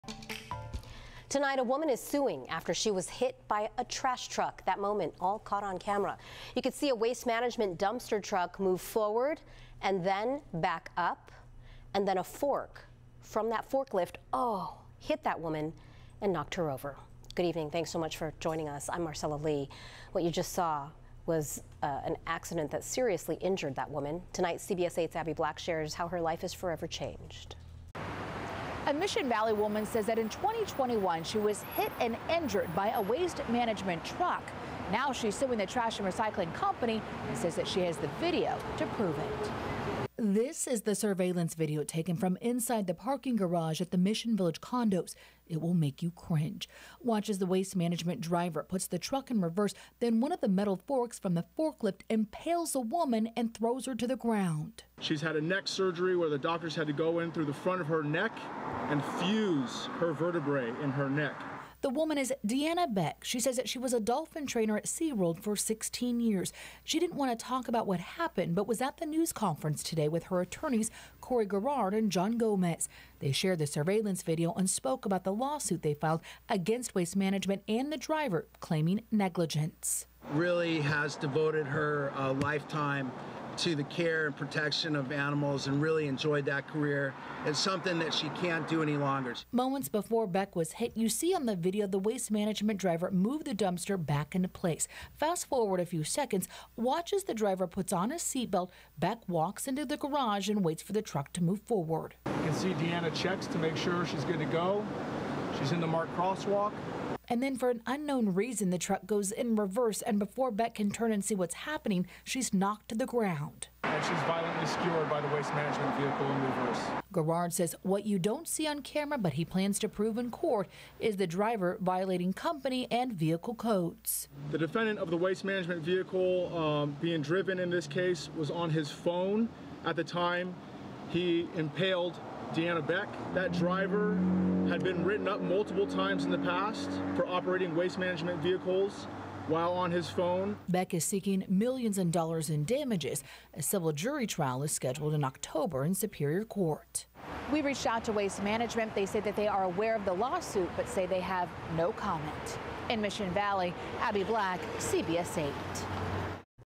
San Diego woman struck by pickup truck sues trash company
A San Diego woman who sustained serious injuries when she was struck by a pickup truck as it backed up at her apartment complex is suing the trash-service company that operated the truck, it was announced Monday.

Deanna Beck was walking through the parking garage at her Mission Valley apartment complex on Feb. 5, 2021 when an employee for Waste Management reversed his truck into her.

Video footage shared by her attorneys shows one of the truck's metal tines -- used for lifting Dumpsters -- striking the woman and sending her to the ground.